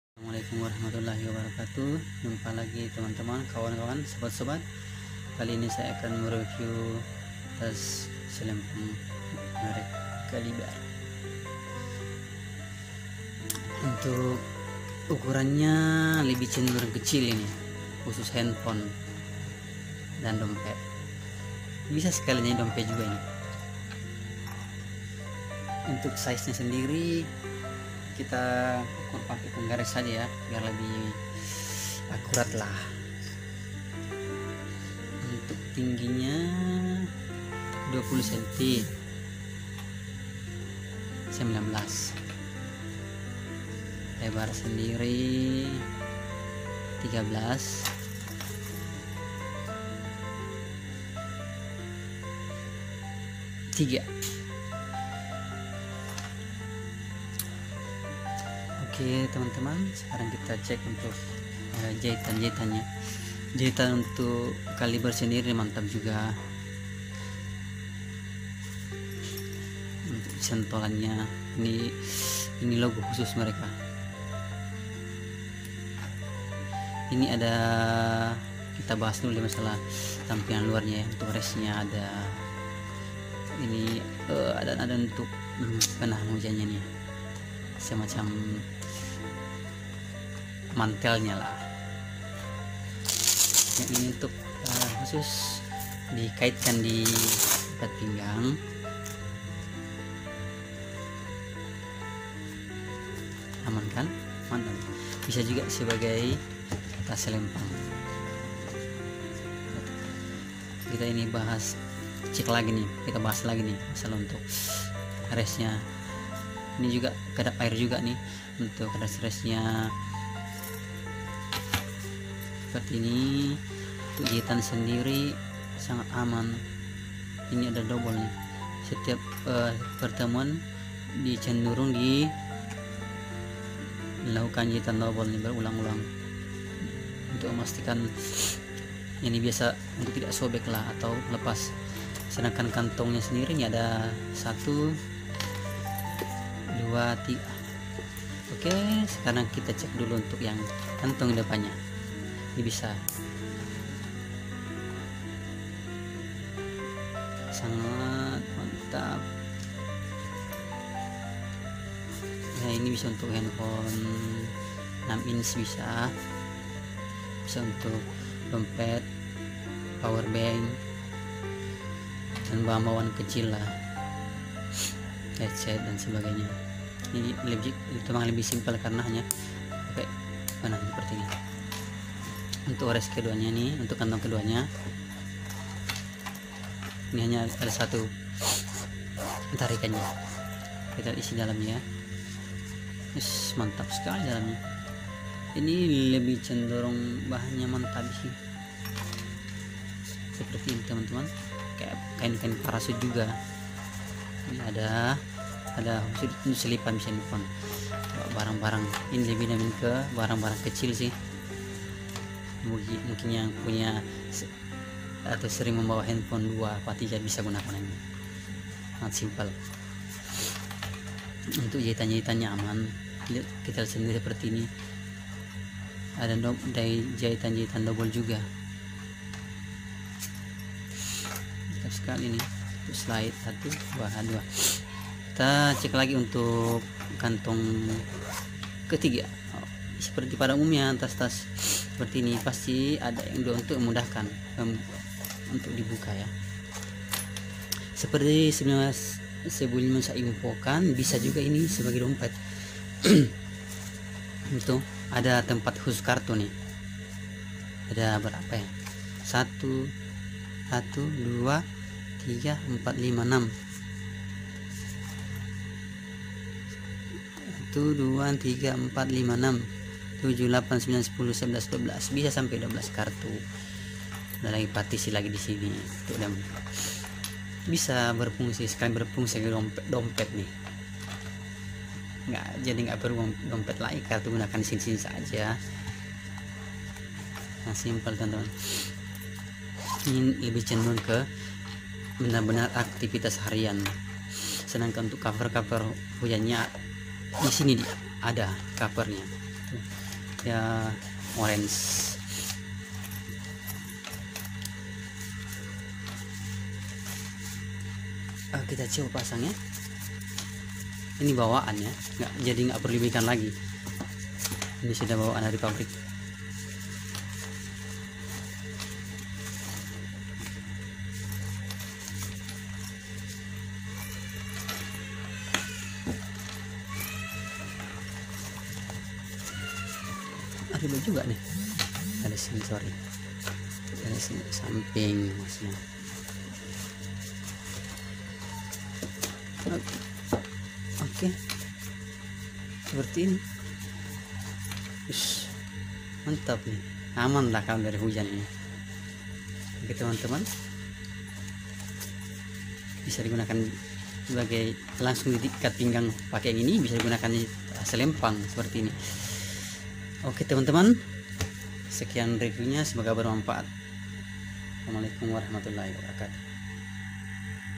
Review Tas Selempang / Sling bag Handphone Smartphone Kalibre 920757-000 - Bigsuanto Angga
* Bisa dijadikan tas selempang dan tas pinggang  (disisipkan diikat pinggang)
* 2 Sekat Handphone 7 Inch
* 12 Sekat Kecil ( kartu)
* Kompartment Power Bank
* Incloude Cover Bag
* Dimensi 20X12X4 Cm